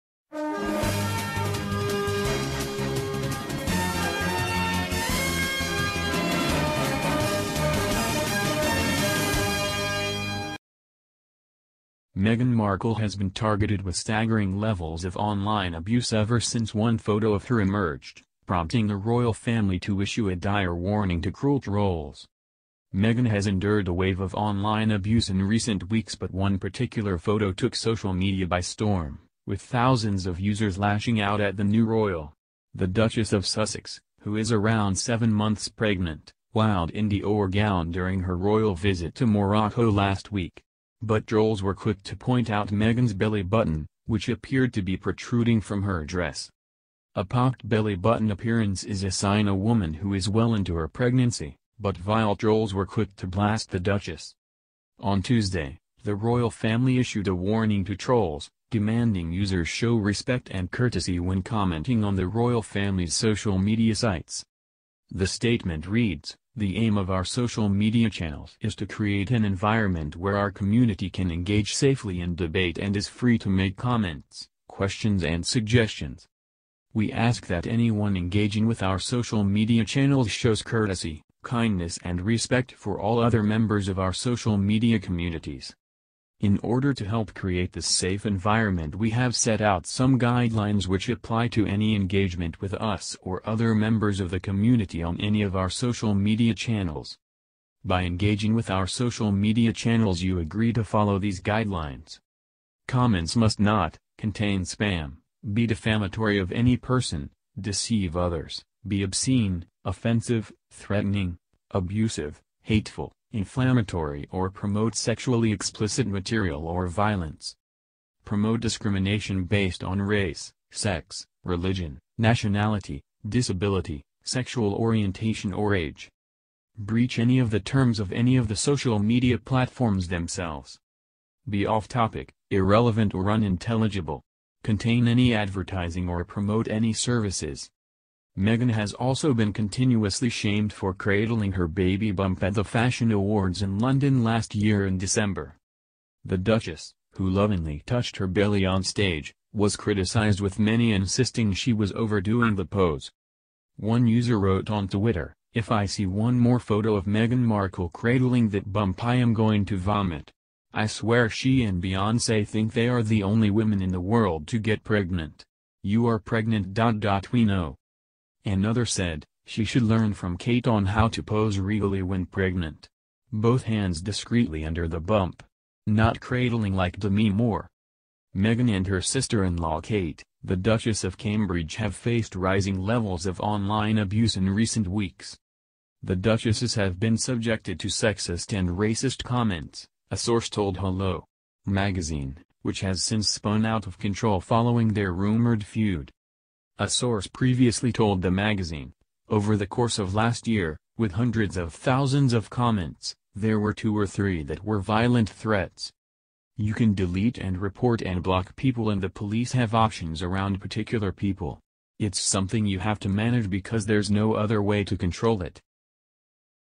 Meghan Markle: This ONE PHOTO sparked such vile abuse Kensington Palace had to step in
MEGHAN Markle has been targeted with staggering levels of online abuse ever since one photo of her emerged, prompting the Royal Family to issue a dire warning to cruel trolls.